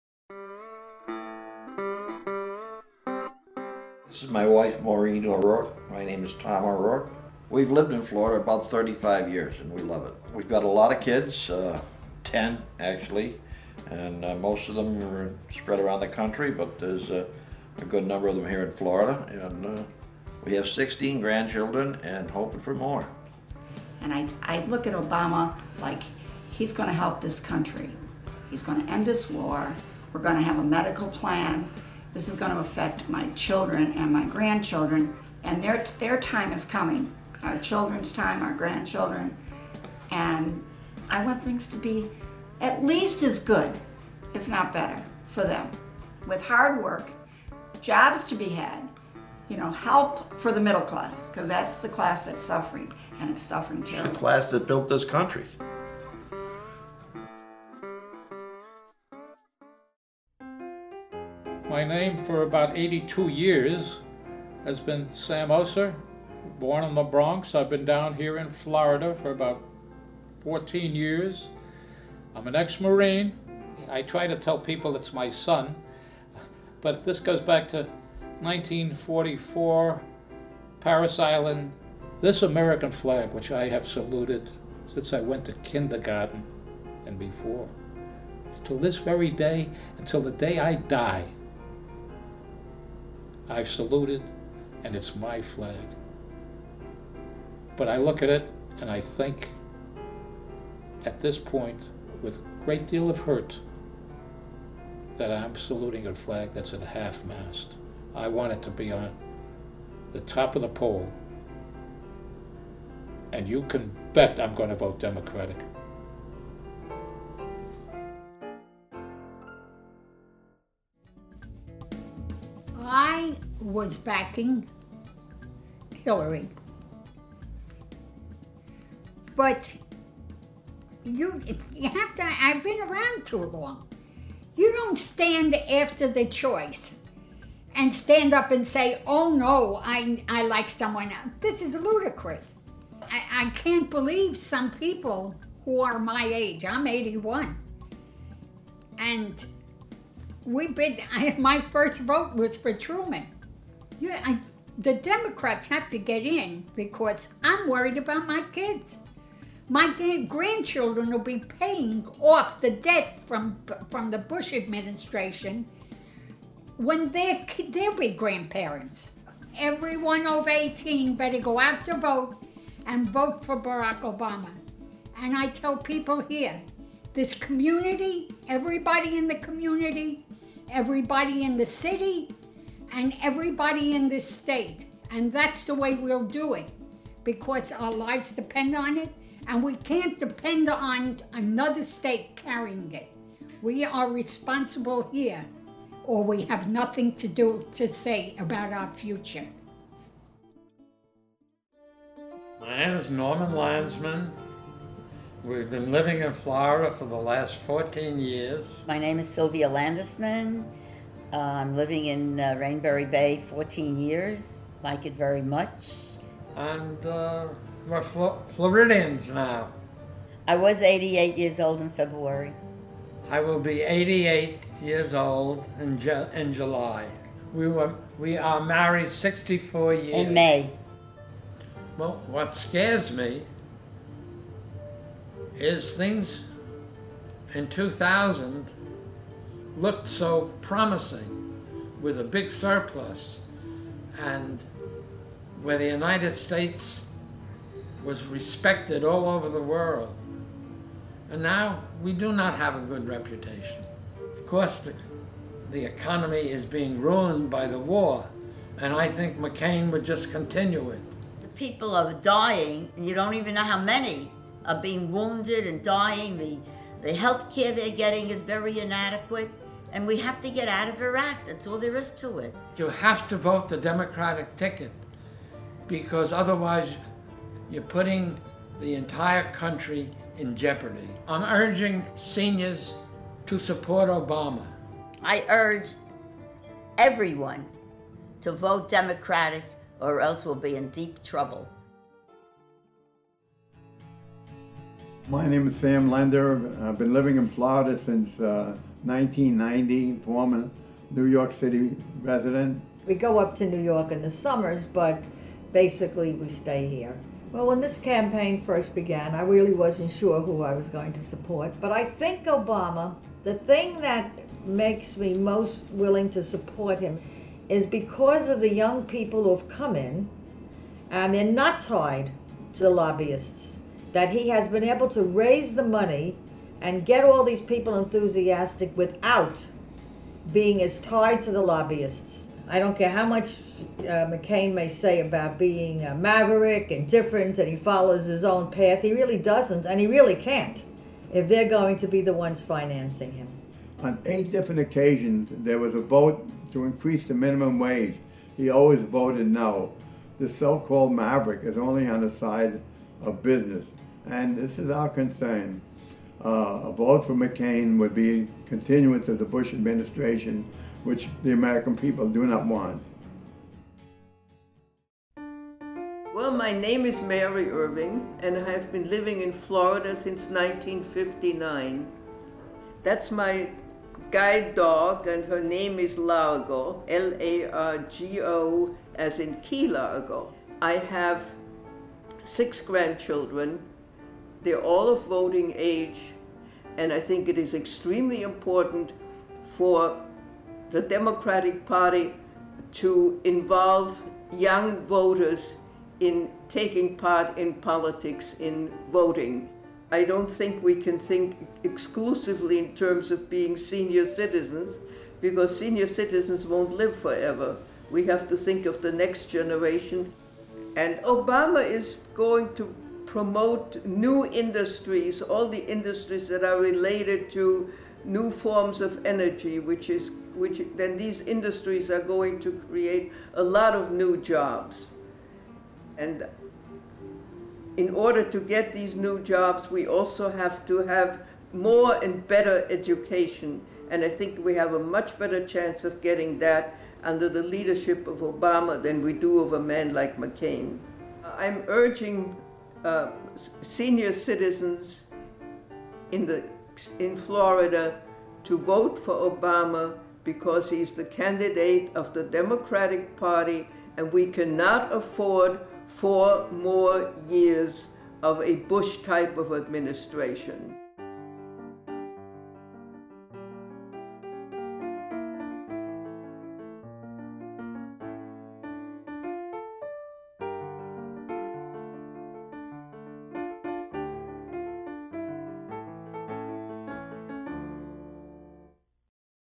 FLORIDA ELDERS FOR OBAMA
First films from eldersforobama.com, a project from Tilson Films Inc., created to help overcome the fears and doubts of many older white voters by cutting through mainstream media and commercial clutter and letting senior Obama supporters speak directly to their peers and to all voters.  No fancy production values or slick graphics, just seniors sitting in their kitchens and living rooms, sharing heartfelt insights about what's at stake in this election.  Please check out the other "Elders On..." films, and keep visiting the website for new films and special guests.